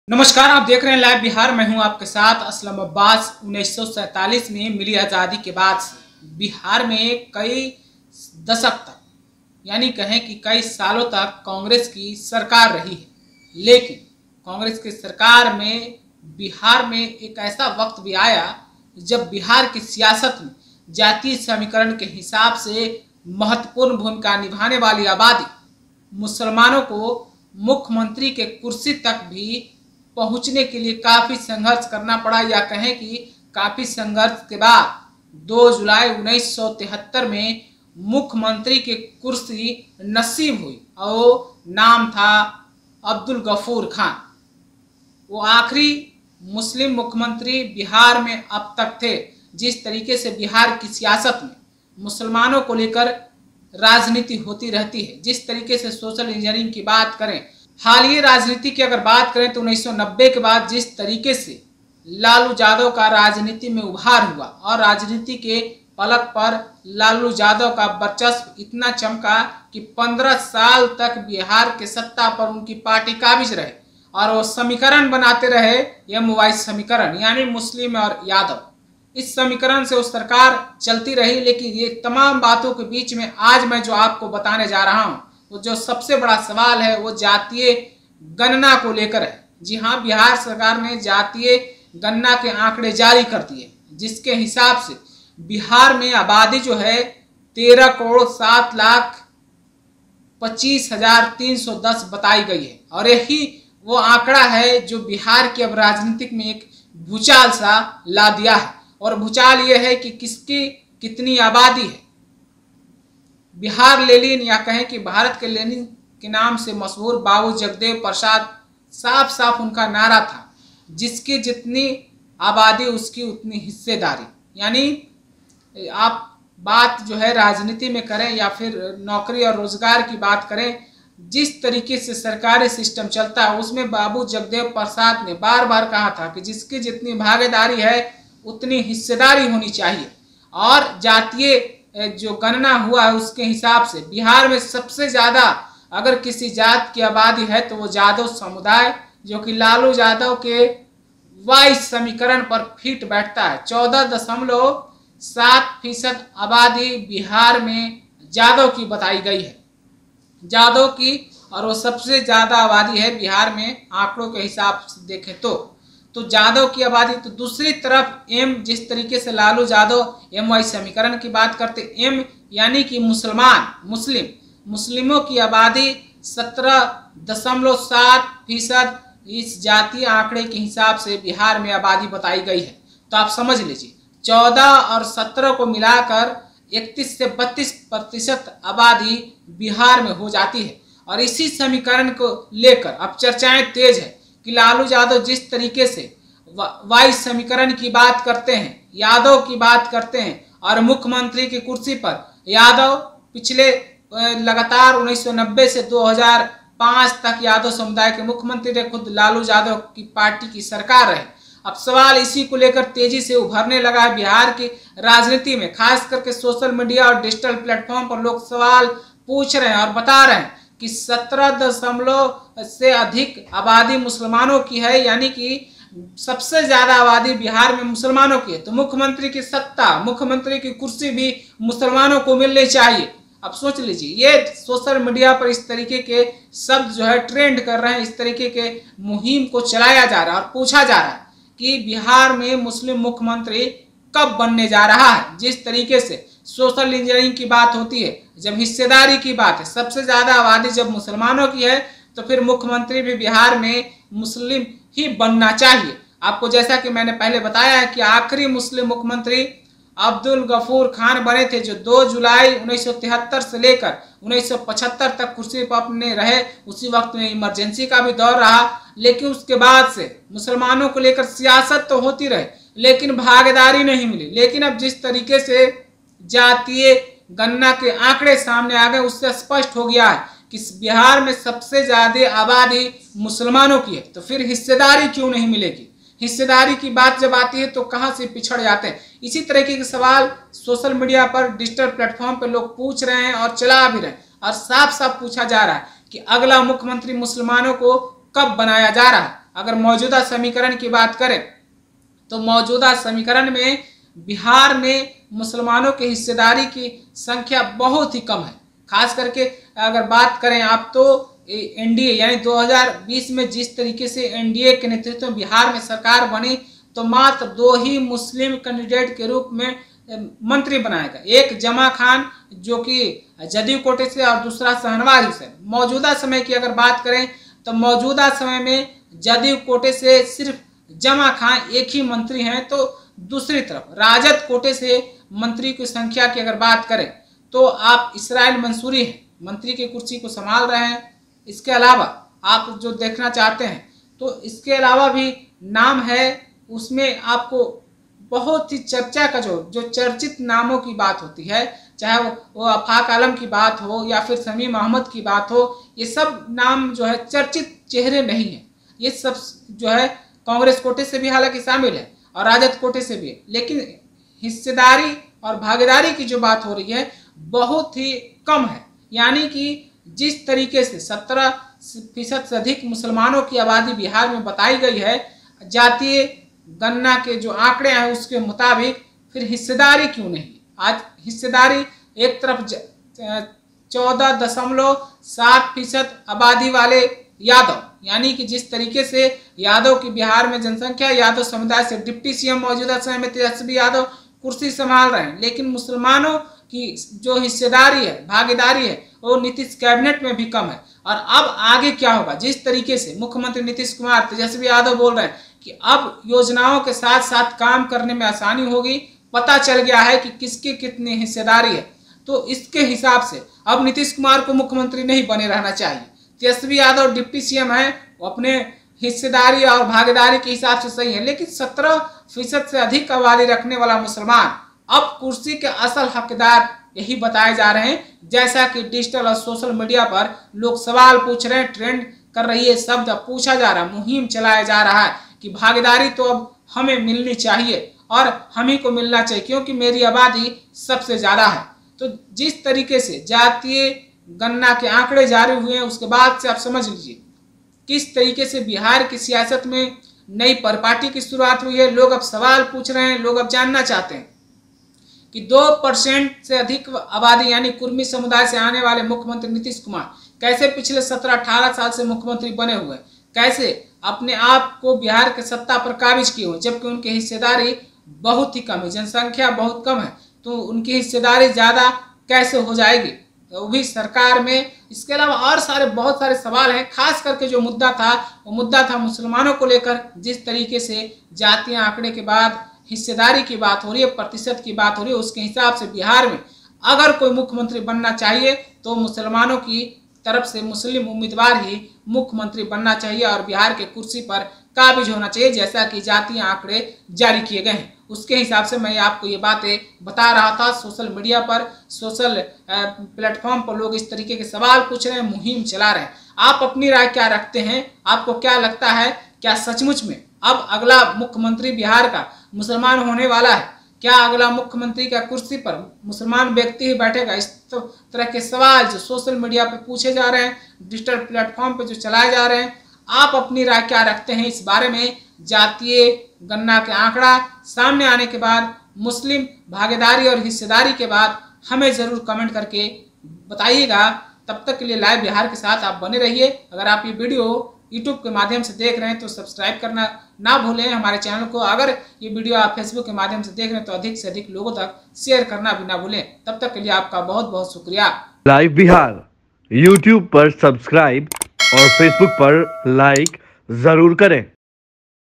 Bihar में अब मुसलमान ही बनेगा CM ? सोशल Media पर मुहिम तेज, जितनी आबादी, उसकी उतनी हिस्सेदारी !
Live Bihar Digital Network